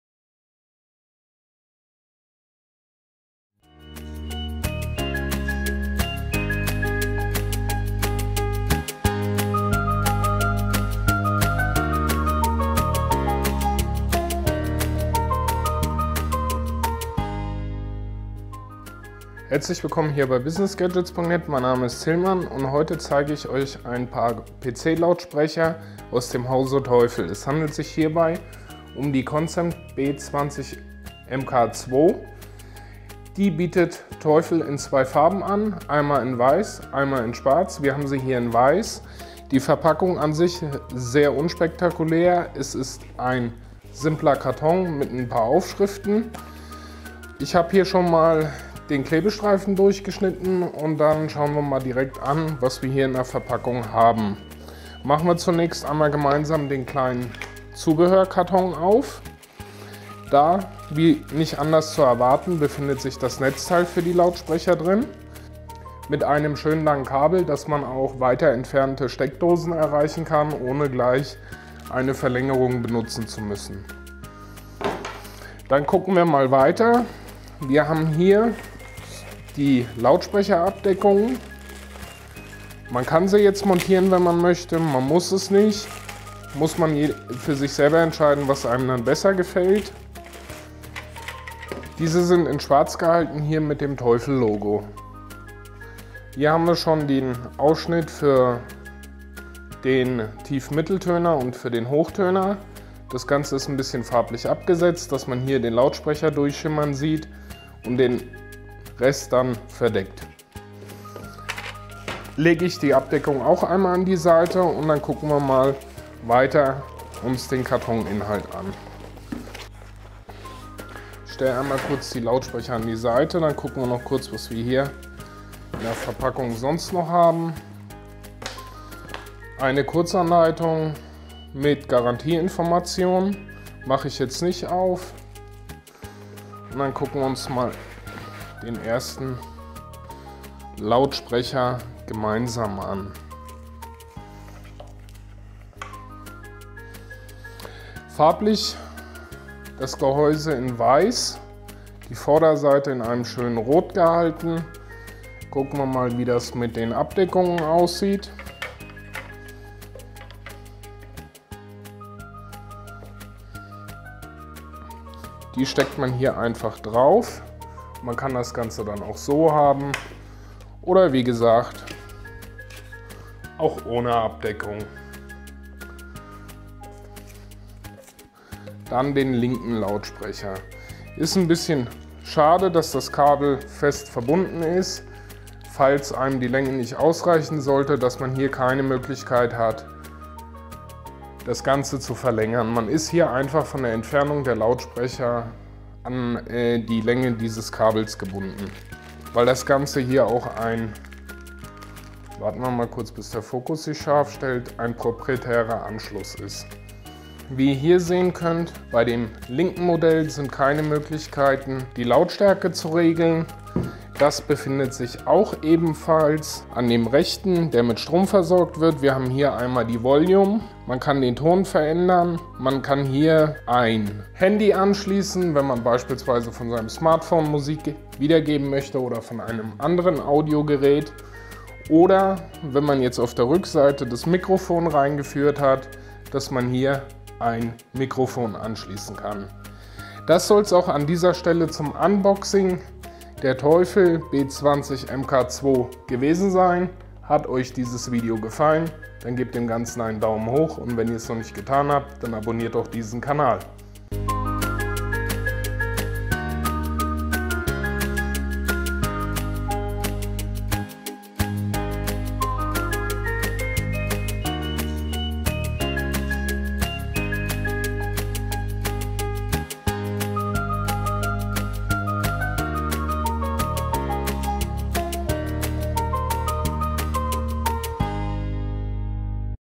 Unboxing Teufel B20MK2 PC Lautsprecher
Heute zeige ich euch das Unboxing der Teufel B20MK2 Computer Lautsprecher.